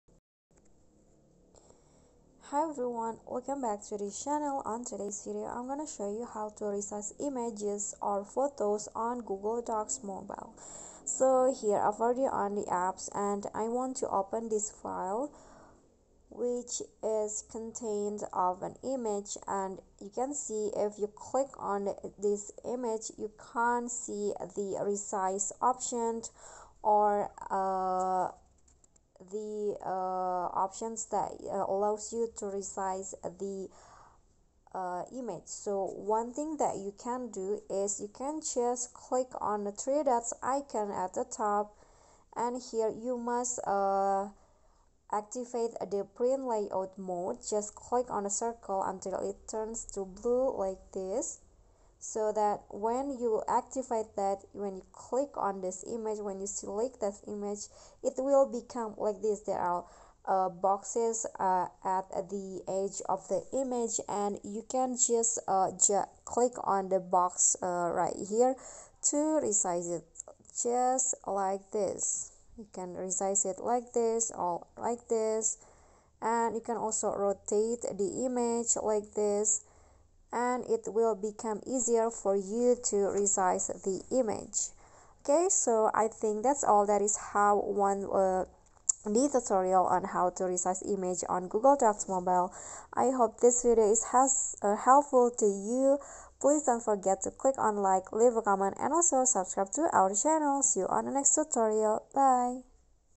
How to Resize Images / Photos on Google Docs Mobile NEW UPDATE August 2022
Here is the solution to you who can't resize images in Google Docs Mobile